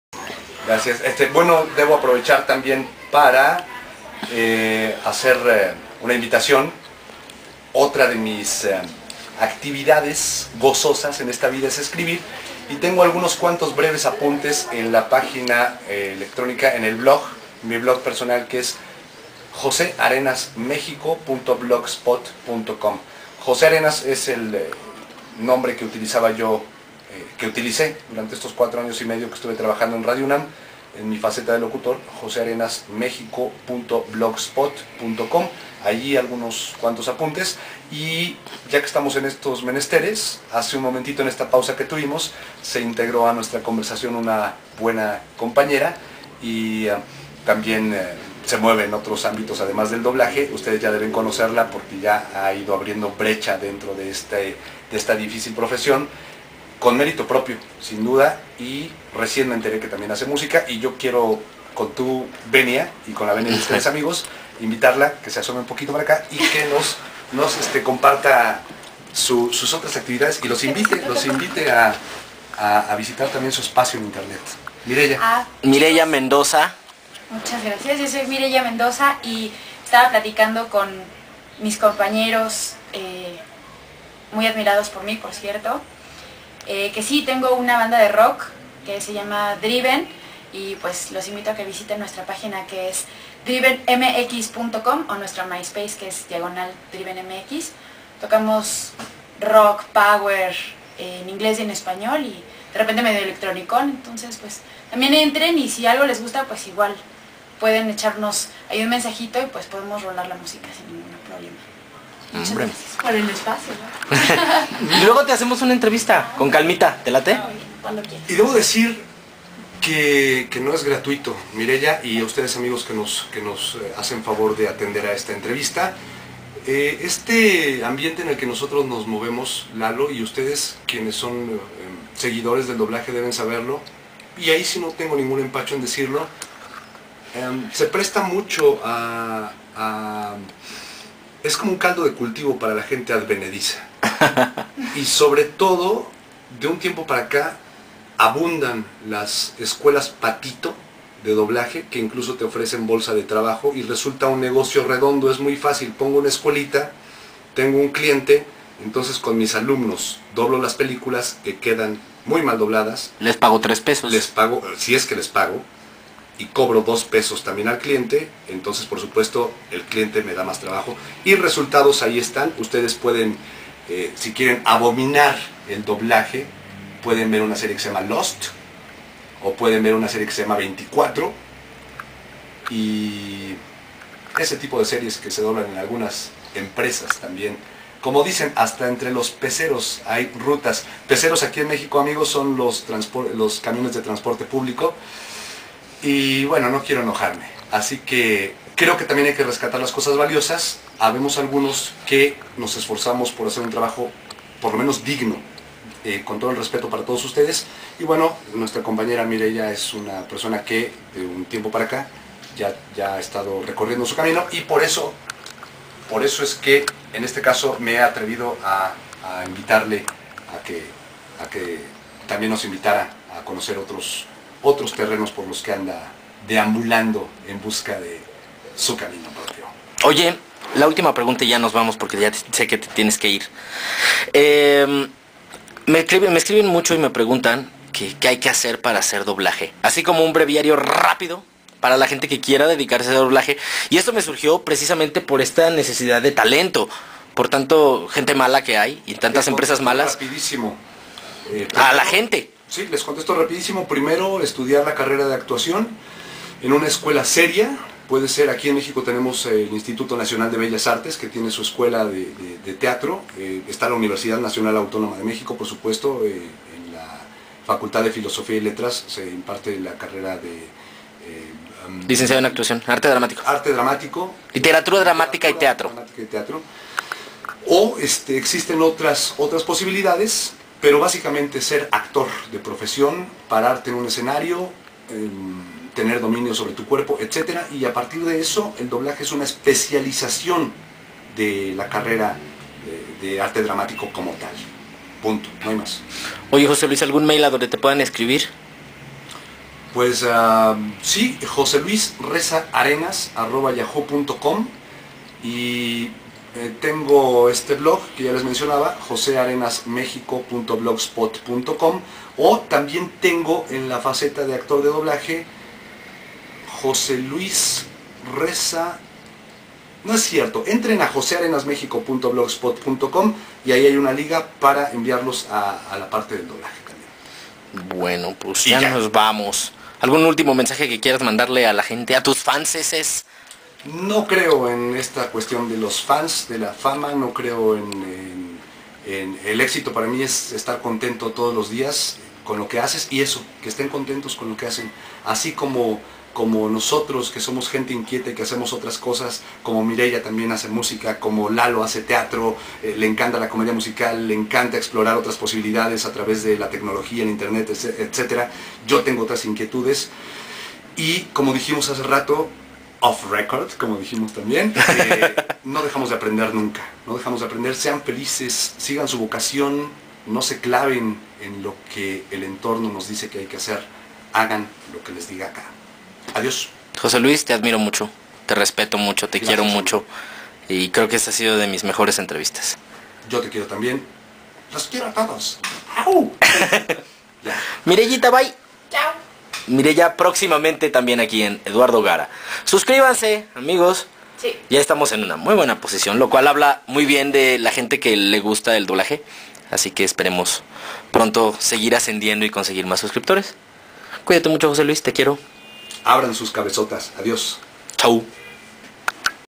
Entrevista a José Luis Reza, voz de Orochimaru en Naruto 3/3
Este hombre es mas que un compañero de trabajo, es un gran amigo y hermano que me ha dado la vida, con un corazón de oro y un criterio inmenso. Entre sus personajes destacan Orochimaru en Naruto, Spencer Shay en Icarly y Eric Foreman de Dr. House. Esta es la tercera parte y última... y NO, TAMPOCO ES LA SORPRESA QUE LES TENGO... PERO YA FALTA MUY POCO.

FE DE ERRATAS:

(José Luis me pidió que publicara esta corrección)

Agradecer a toda la gente que, gentilmente, se ha tomado la molestia de hacer un comentario respecto a la entrevista que me hiciste. Y aclarar un par de imprecisiones en que incurrí: La esposa de Robert Schumann se llama Clara, no Ana como dije. Y el violonchelista Yo-Yo-Ma, es norteamericano, de padres chinos, no japonés como, de manera irresponsable, aseguré. Un abrazo.